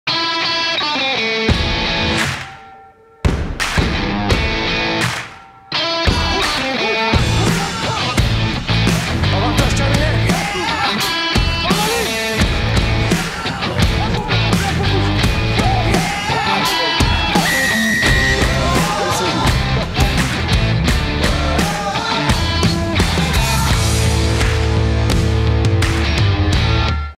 L'Action du Match SPASM by Paprec
Retour en Images sur l'essai du local de cette étape paloise du Top 14, Bastien Pourailly qui se débarrasse de son défenseur par un magnifique crochet intérieur après une fixation parfaite de Wesley Fofana mis sur orbite par une longue passe de Camille Lopez. L'ailier clermontois inscrit son premier essai en "jaune et bleu" sur la pelouse où il a été formé, après un beau plongeon pour éliminer les retours défensifs.